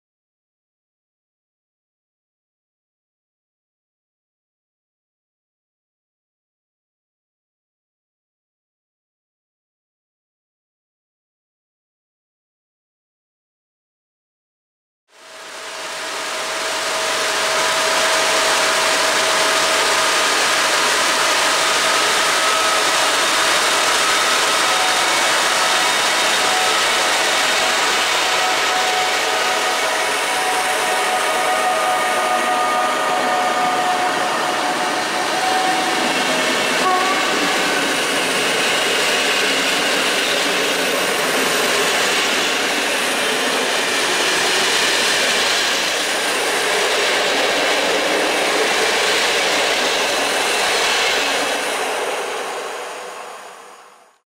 EU160-030 "Produkt Polski" KupujŚwiadomie z IC 3700/1 Ślązak do Poznania Głównego. +RP1
EU160-030 Newag Griffin w okleinie Produkt Polski KupujŚwiadomie na czele pociągu IC 3700/1 Ślązak relacji Przemyśl Główny - Poznań Główny odjeżdża ze stacji Żmigród.

Województwo: Dolnośląskie
Miejscowość: Żmigród
Data: 19 Luty 2021r.
Godzina: 21:13
Przewoźnik: PKP Intercity
D29: 271 (Wrocław-Poznań)
Montaż: Camtasia Studio